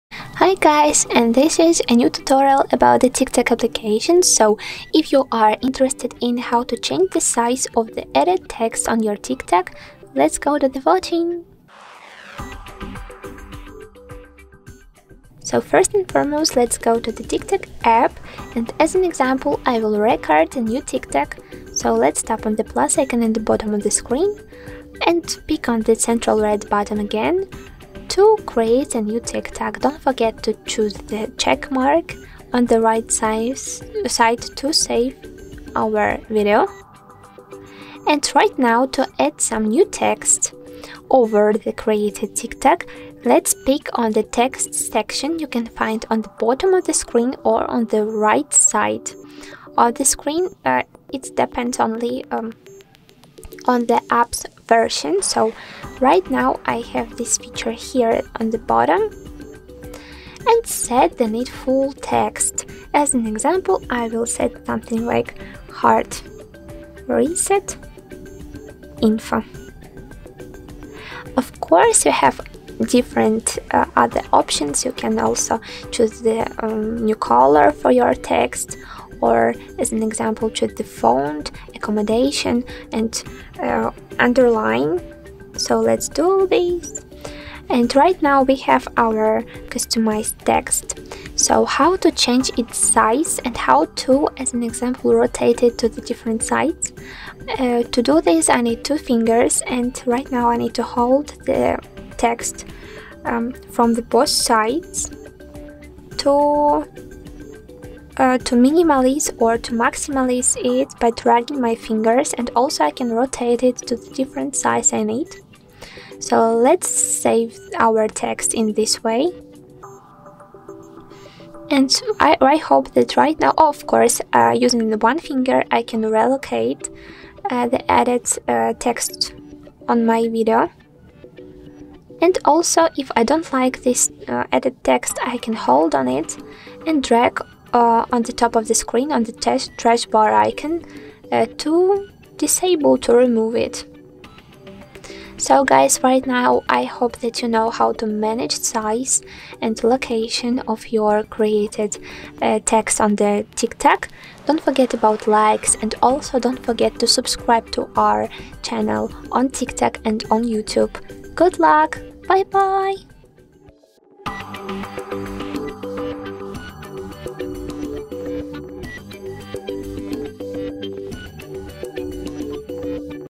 How to Change Added Text Size on Tik Tok
Hi there! Have you ever been wondering how you can change added text size in Tik Tok application? In today's video we're going to show you how you can change added text size on your SONY Xperia 1 IV just in a couple of simple steps. So if you wish to learn how you can adjust text size in Tik Tok application on your SONY Xperia 1 IV, all you need to do is just watch this short video to the end, follow the instructions provided by our specialist and try to perform this operation yourself on your smartphone.

How to change text size in Tik Tok? How to adjust text in Tik Tok? Hot to edit text settings in Tik Tok?